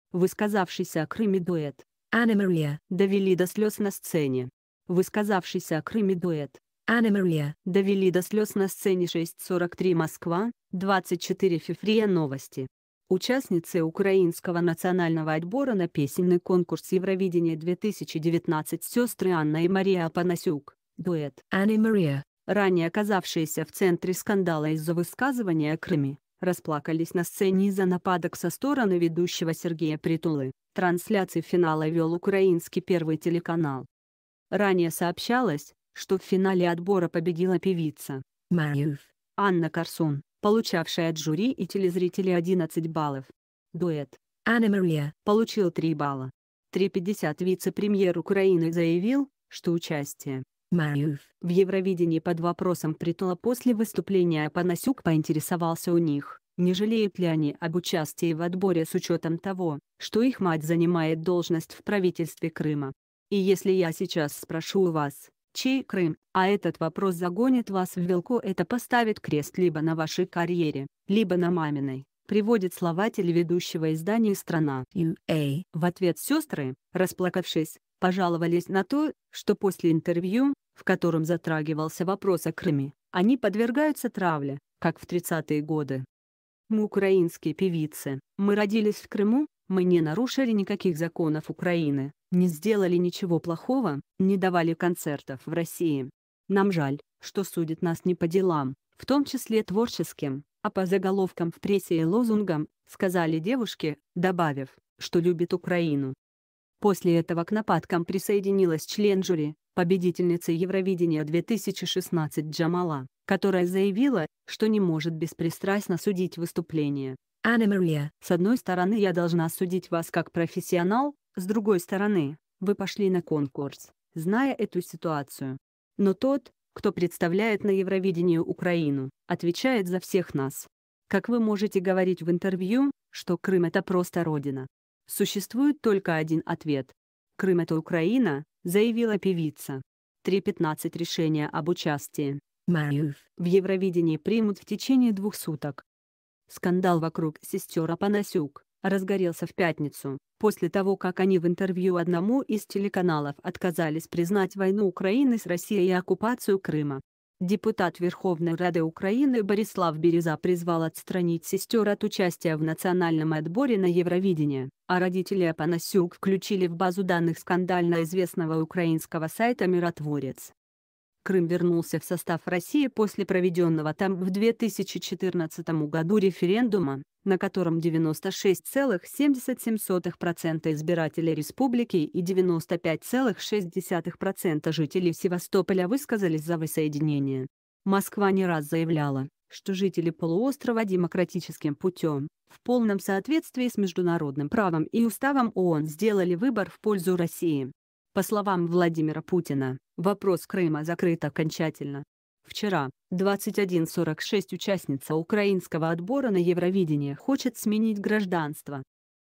Высказавшийся о Крыме дуэт Anna Maria довели до слез на сцене
Высказавшийся о Крыме дуэт Anna Maria довели до слез на сцене Высказавшийся о Крыме дуэт Anna Maria довели до слез на сцене 06 43 МОСКВА, 24 фев РИА Новости . Участницы украинского национального отбора на песенный конкурс Евровидение-2019 сестры Анна и Мария Опанасюк дуэт Anna Maria , ранее оказавшиеся в центре скандала из-за высказываний о Крыме, расплакались на сцене из-за нападок со стороны ведущего Сергея Притулы, трансляцию финала вел украинский Первый телеканал . Ранее сообщалось, что в фин